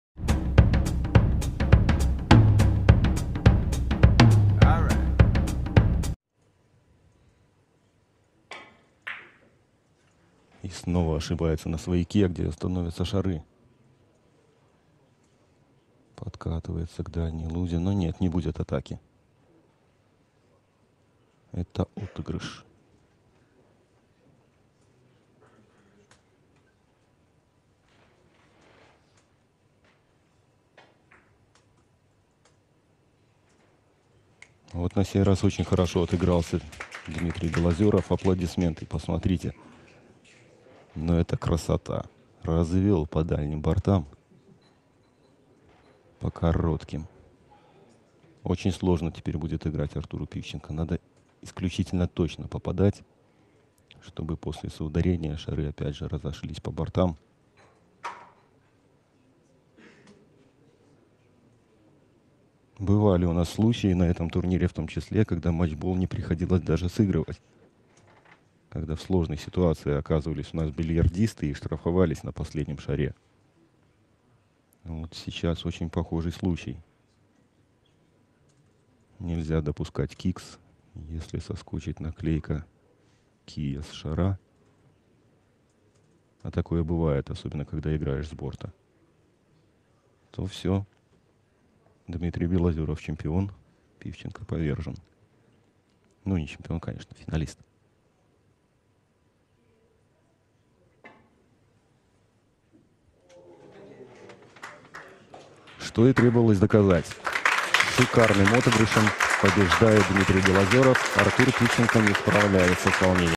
Победа шикарным отыгрышем. Белозеров - Пивченко.
Шикарный отыгрыш от Дмитрия Белозерова в контре на 7:7.